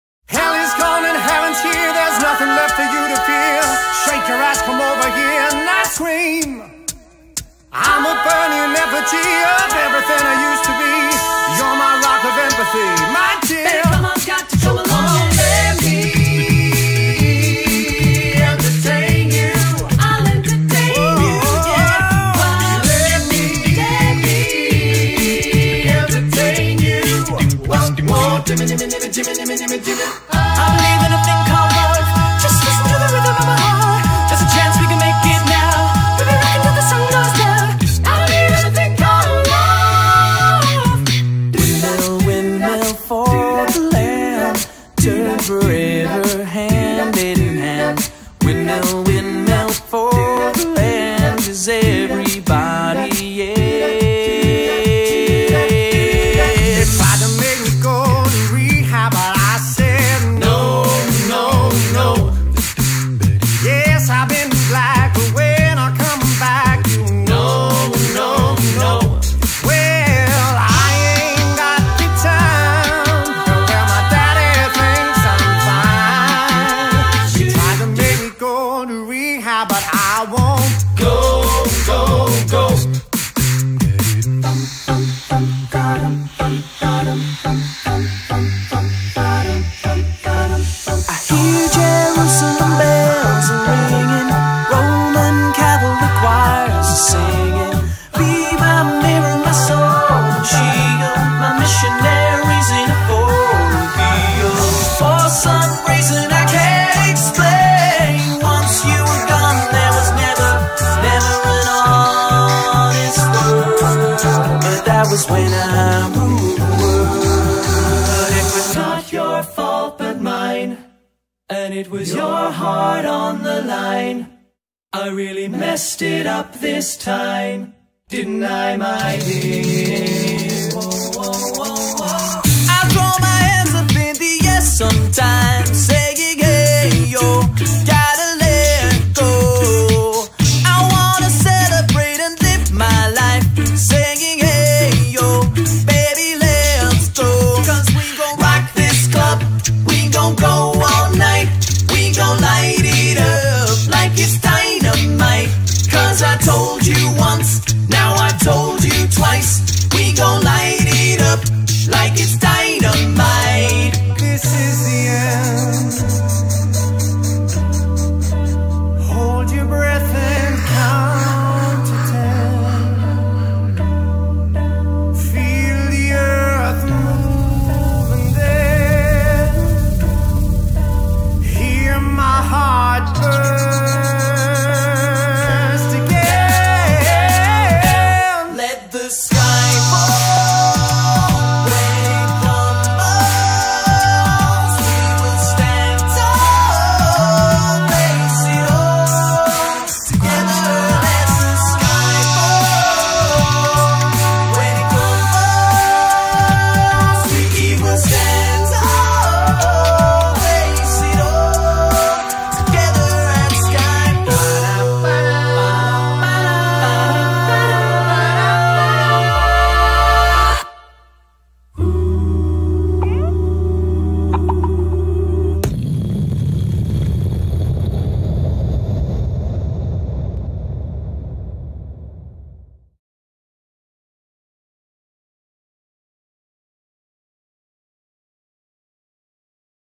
A Capella Beatbox group | Best of British Pop
Unique, unforgettable entertainment!

Give your guests an event to remember with entertainment from this truly unique group. Combining 4 powerful singing voices and an incredible beatboxer, your audience will be amazed at the melodic mastery of this group - you won’t find any backing track or special effects here - just pure talent and an incredible craft.